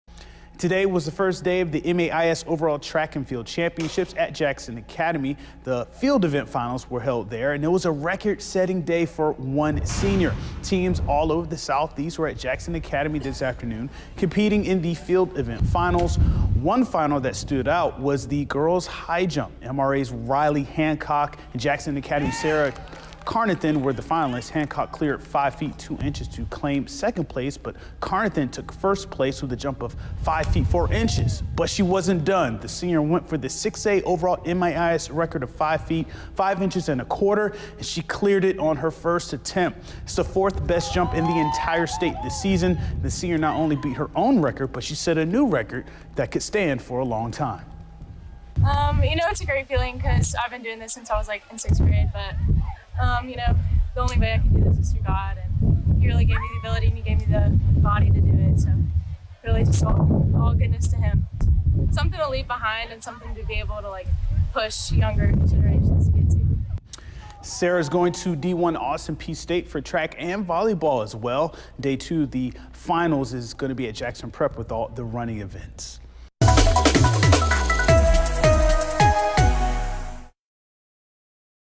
Jackson Academy senior sets new MAIS record in the high jump
Jackson Academy senior high jumper Sara Carnathan put her name in the record books in her last track meet for the Raiders.  Sara set the a new 6A Overall MAIS record clearing 5 feet, 5 inches and a quarter in the girls high jump event. It's the fourth best jump in the entire state this season. She also won the overall state title clearing five-feet and four inches.  The senior is headed to Division 1 Austin Peay State to continue to run track and play volleyball. She credits God for her success and hopes her mark inspires the next generation of high jumpers in the MAIS.